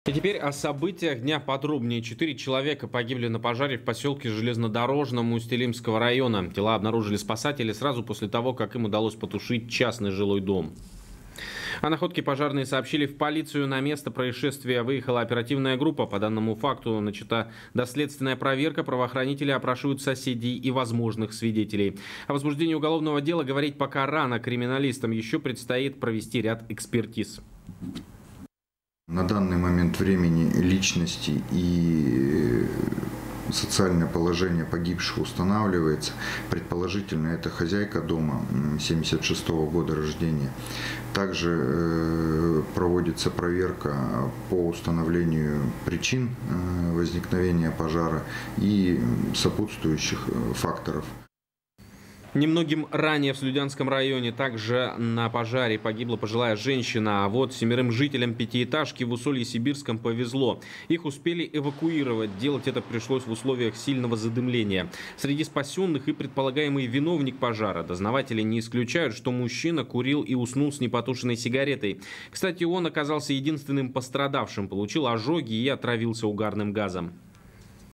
Четыре человека погибли на пожаре в Усть-Илимском районе
Четыре человека погибли на пожаре в посёлке Железнодорожном Усть-Илимского района. Тела обнаружили спасатели сразу после того, как им удалось потушить частный жилой дом.

О находке пожарные сообщили в полицию. На место происшествия выехала оперативная группа. По данному факту начата доследственная проверка. Правоохранители опрашивают соседей и возможных свидетелей. О возбуждении уголовного дела говорить пока рано, криминалистам ещё предстоит провести ряд экспертиз.

ИВАН НИКИТЧЕНКО, начальник отдела надзорной деятельности по Усть-Илимску: "На данный момент времени личности и социальное положение погибших устанавливается. Предположительно, это хозяйка дома 76 года рождения. Также проводится проверка по установлению причин возникновения пожара и сопутствующих факторов".

Немногим ранее в Слюдянском районе также на пожаре погибла пожилая женщина. А вот семерым жильцам пятиэтажки в Усолье-Сибирском повезло. Их успели эвакуировать. Делать это пришлось в условиях сильного задымления. Среди спасённых – и предполагаемый виновник пожара. Дознаватели не исключают, что мужчина курил и уснул с непотушенной сигаретой. Кстати, он оказался единственным пострадавшим - получил ожоги и отравился угарным газом.